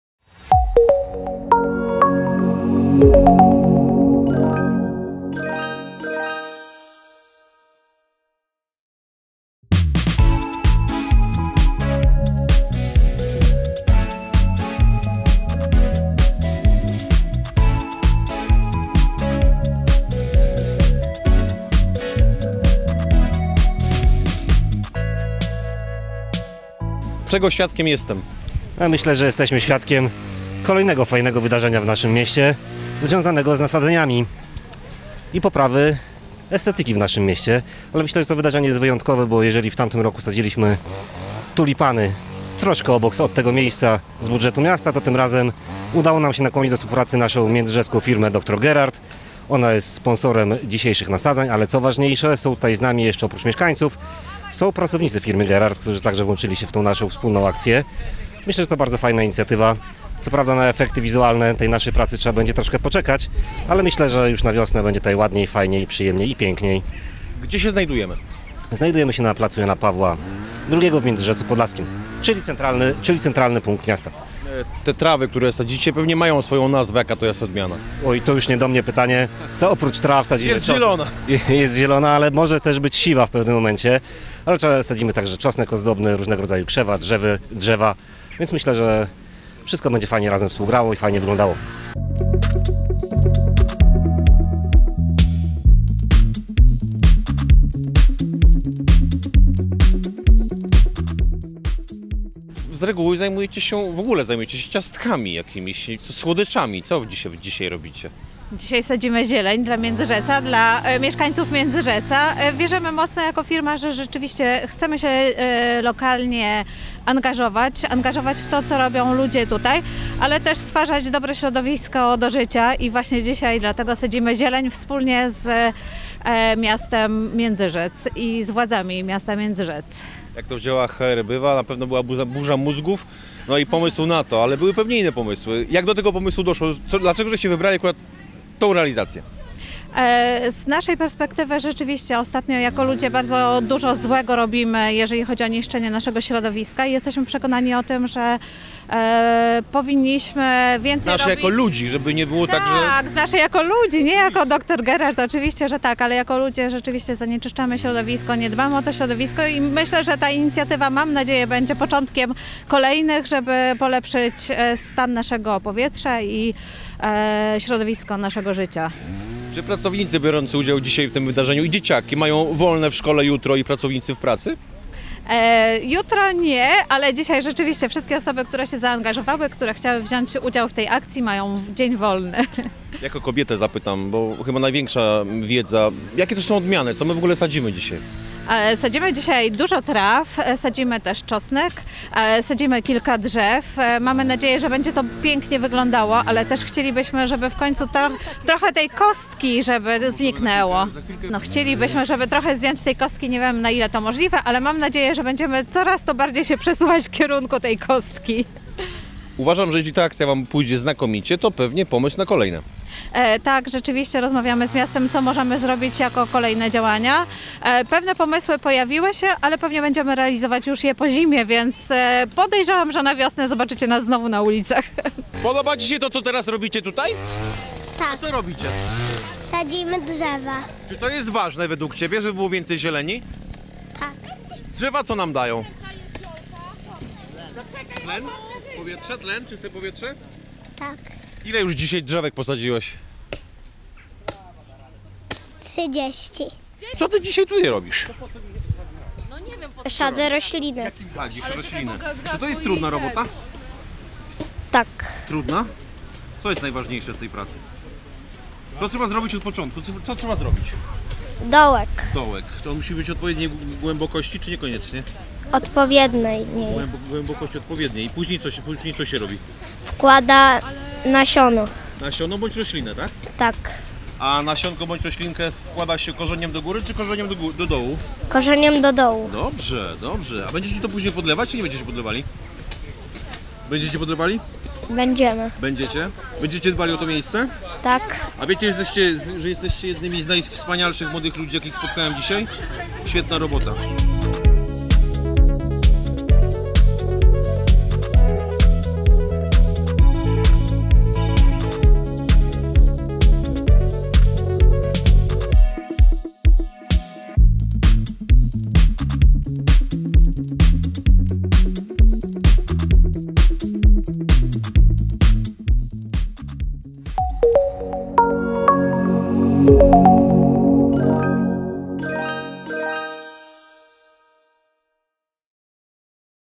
Dozielenienie placu Jana Pawła II w Międzyrzecu Podlaskim pod hasłem "Chodźmy w zielone"
Miasto Międzyrzec Podlaski wspólnie z firmą Dr Gerard zaprosiło w dniu 20 października swoich mieszkańców do udziału w akcji dozielenienia placu Jana Pawła II pod hasłem "Chodźmy w zielone".